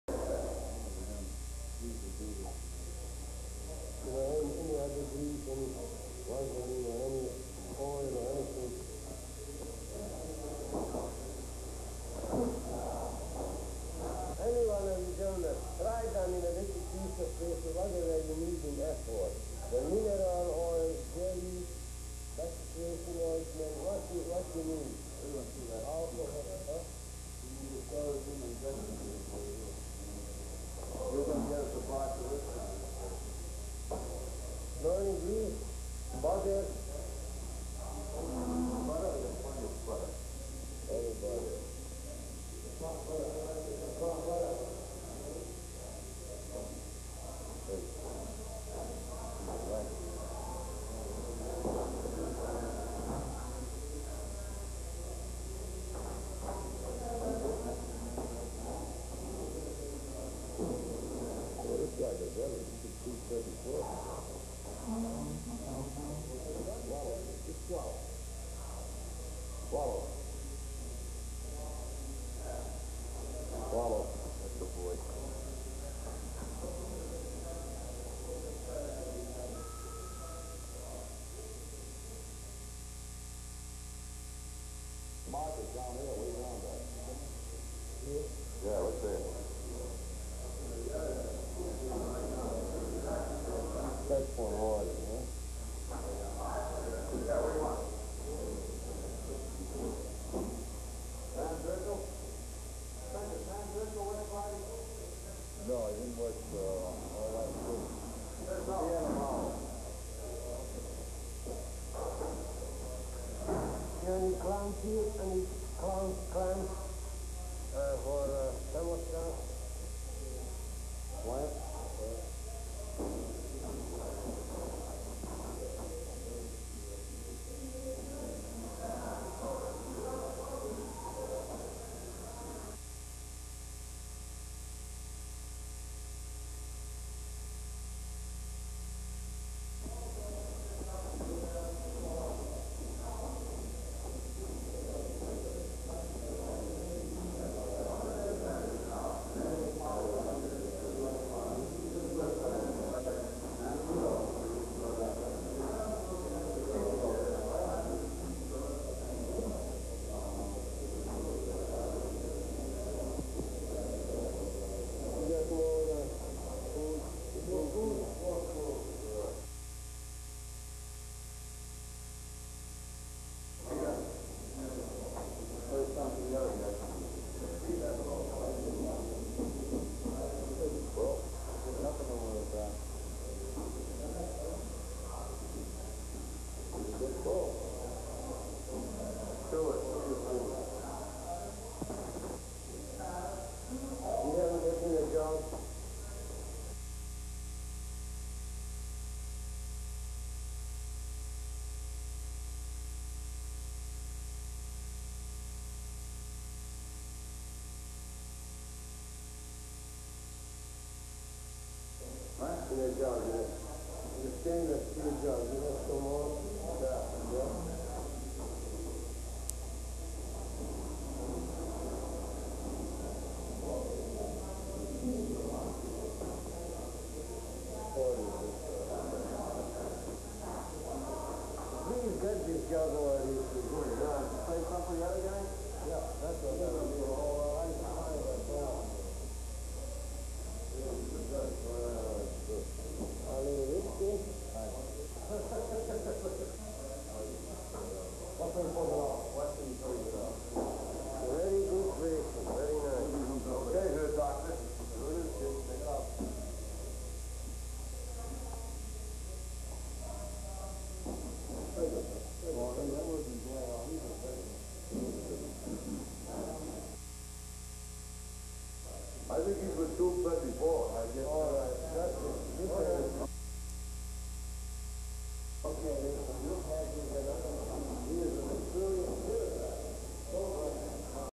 Warning Disturbing Video Of Man Being Force Fed At Mental Hospital
A man being fed through a rubber hose and another man being prepared by hospital mortician. This is disturbing and should not be watched  by sensitive viewers.  20th century psychiatric hospital documentary.